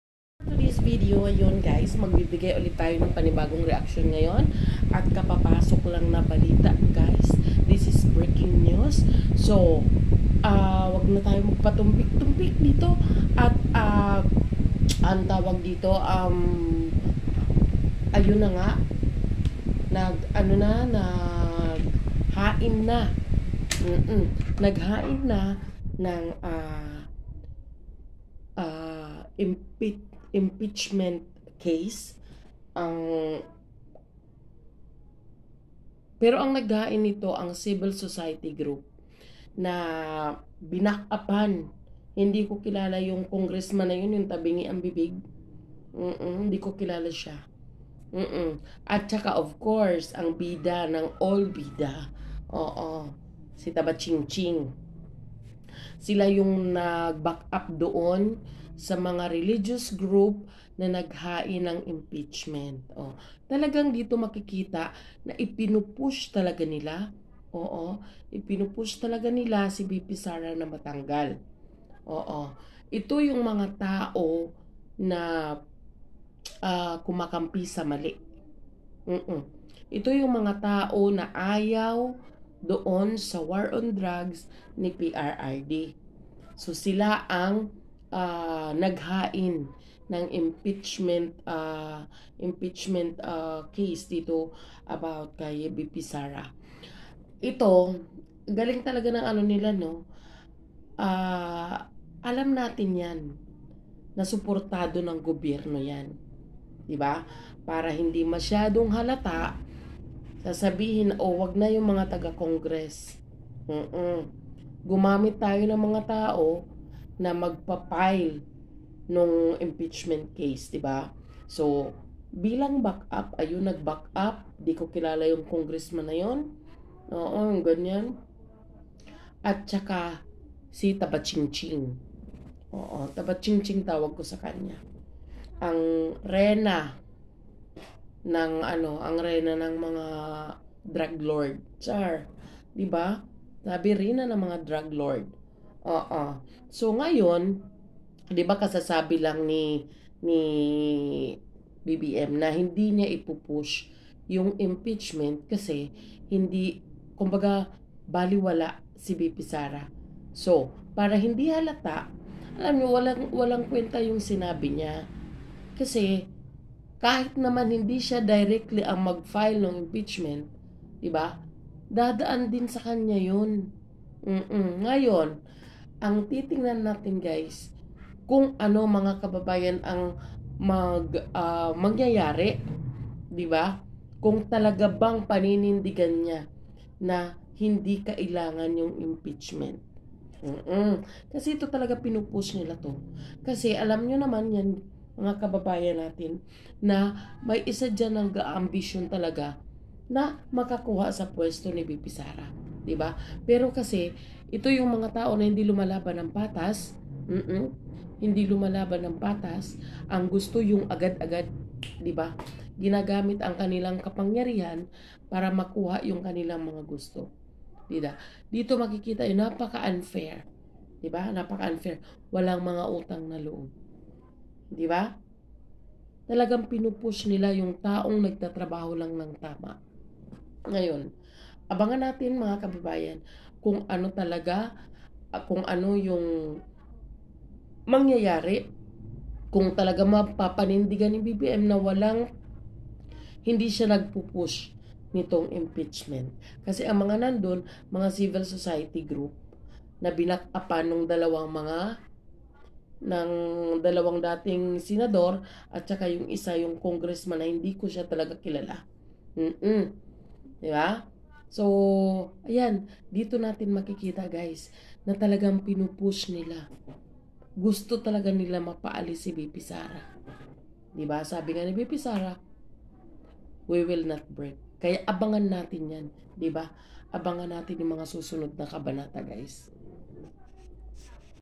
ANU BA PAGKATAO NG MGA NAGHAIN NG IMPEACHMENT /SILA ANG DAPAT NAWALA
Reaction lang po at opinyon